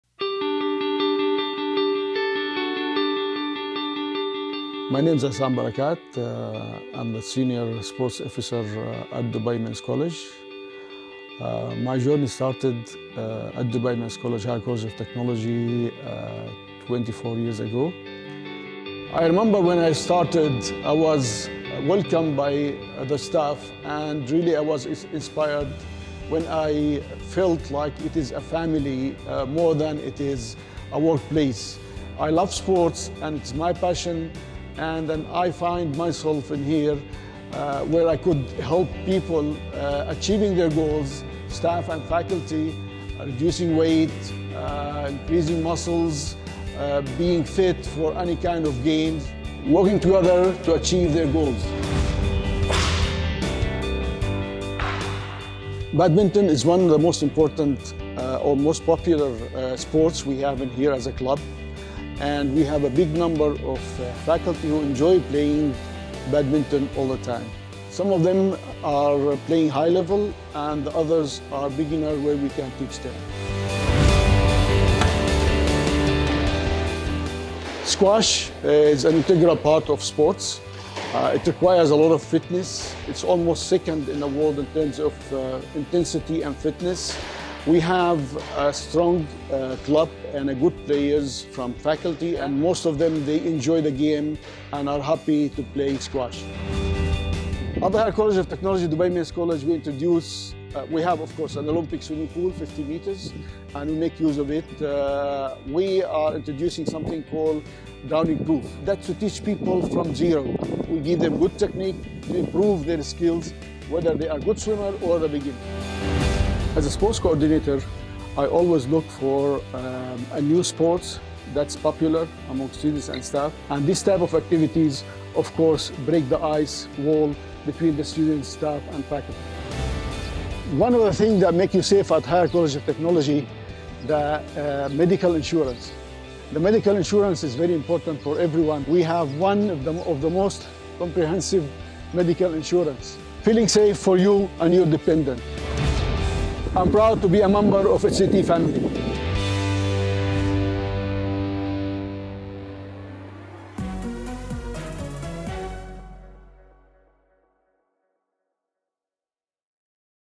Meet Essam Barakat.. 24-year HCT employee who helps people achieve their goals
Inspiration, Drive & Passion come in all forms. Meet Issam Barakat, a 24-year HCT employee who helps people achieve their goals, through his love of sports, as part of the dynamic and diverse HCT family. Join our HCT family to unlock and share your unique spirit